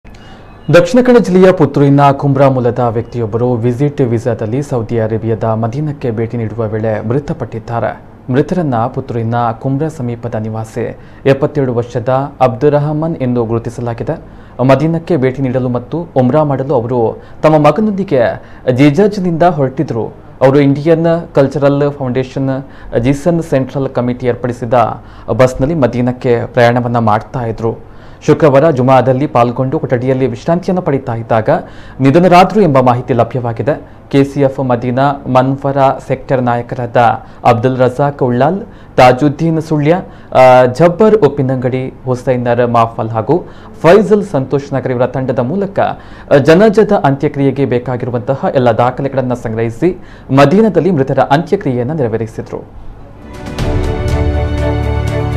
ಉಮ್ರಾ ಯಾತ್ರೆ ಗೆ ತೆರಳಿದ್ದ ಪುತ್ತೂರು ಕುಂಬ್ರದ ವ್ಯಕ್ತಿ ಮದೀನದಲ್ಲಿ ನಿಧನ..!
ದಕ್ಷಿಣ  ಕನ್ನಡ ಜಿಲ್ಲೆಯ ಪುತ್ತೂರಿನ   ಕುಂಬ್ರ ಮೂಲದ ವ್ಯಕ್ತಿಯೊಬ್ಬರು ವಿಸಿಟ್ ವೀಸಾದಲ್ಲಿ  ಸೌದಿ ಅರೇಬಿಯ  ಮದೀನಾಕ್ಕೆ  ಭೇಟಿ ನೀಡುವ  ವೇಳೆ ಮೃತಪಟ್ಟಿದ್ದಾರೆ.